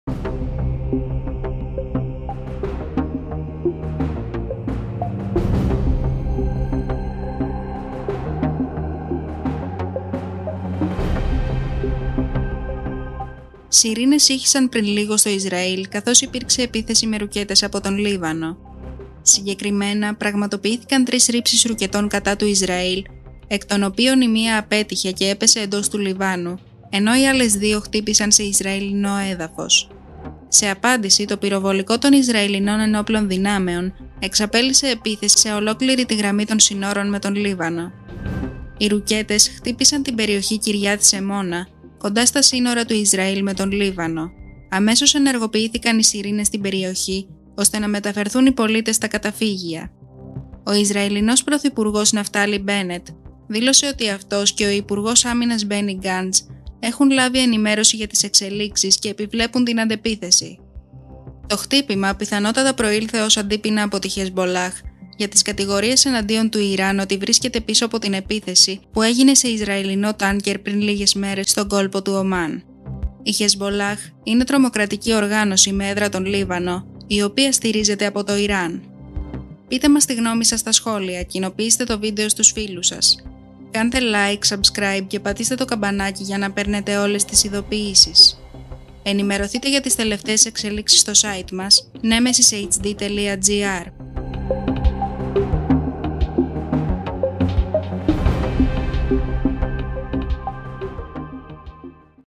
ΕΚΤΑΚΤΟ: Το Ισραηλινό πυροβολικό σφυροκοπά τον Λίβανο - Βολές ρουκετών
📰 ΙΣΡΑΗΛ - Σειρήνες ήχησαν πριν λίγο στο Ισραήλ καθώς υπήρξε επίθεση με ρουκέτες από τον Λίβανο. Δες την είδηση

CHAPTERS

00:00​​​ INTRO
00:10​​​ ΕΚΤΑΚΤΟ: Το Ισραηλινό πυροβολικό σφυροκοπά τον Λίβανο - Βολές ρουκετών
01:35​​​ OUTRO